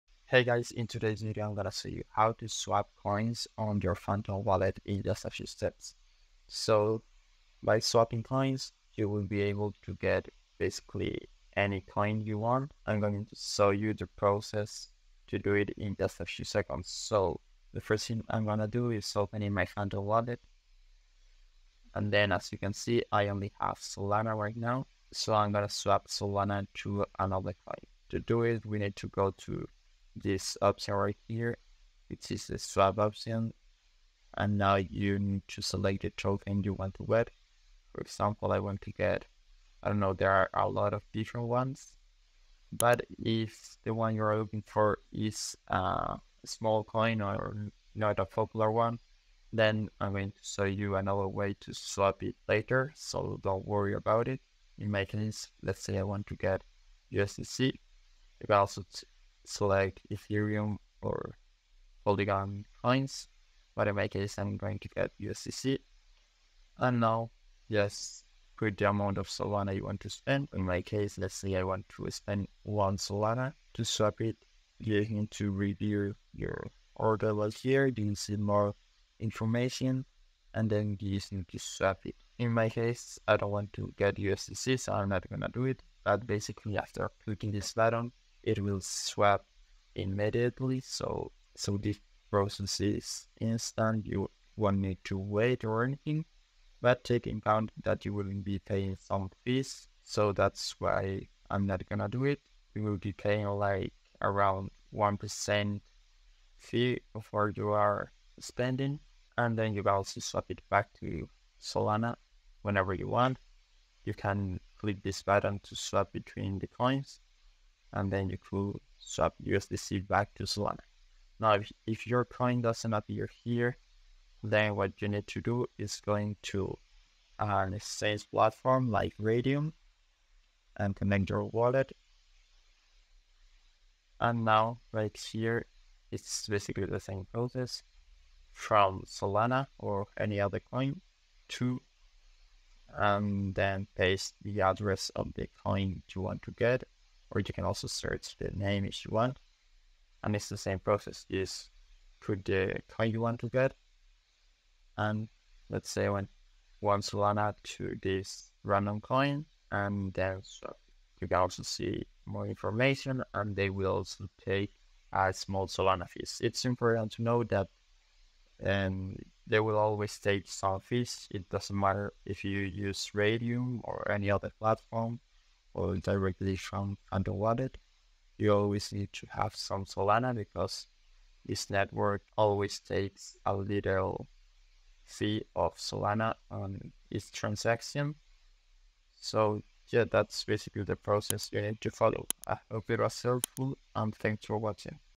How to Swap Coins on Phantom Wallet - 2025 Tutorial
Easy tutorial to Swap or Convert coins or tokens from your phantom wallet step by step! Learn how to buy any coin on your phantom wallet in just a few seconds with this full guide, buy solana, ethereum or polygon coins